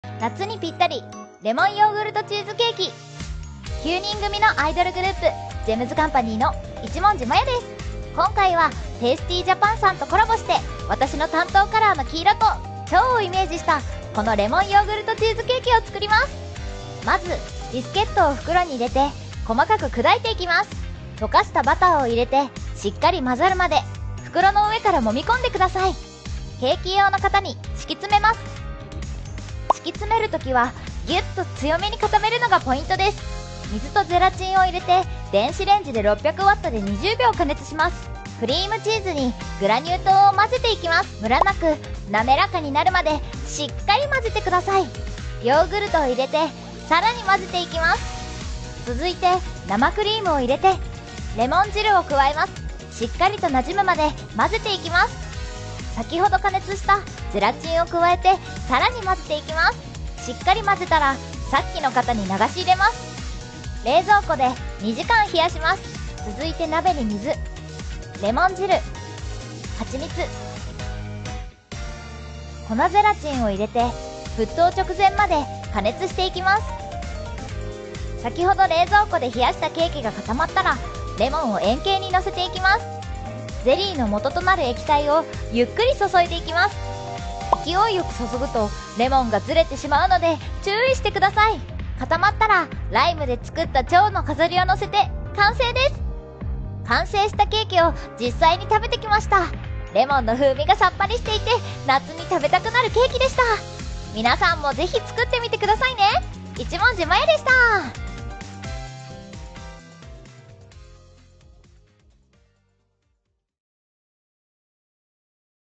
Refreshing Lemon Yogurt Cheesecake Recipe! Collaborating with Tasty Japan
In this refreshing Lemon Yogurt Cheesecake Recipe, I team up with the incredible talents from Tasty Japan to bring you a mouthwatering dessert that will leave you craving for more! Watch as we combine creamy yogurt and zesty lemons to create the perfect summer treat.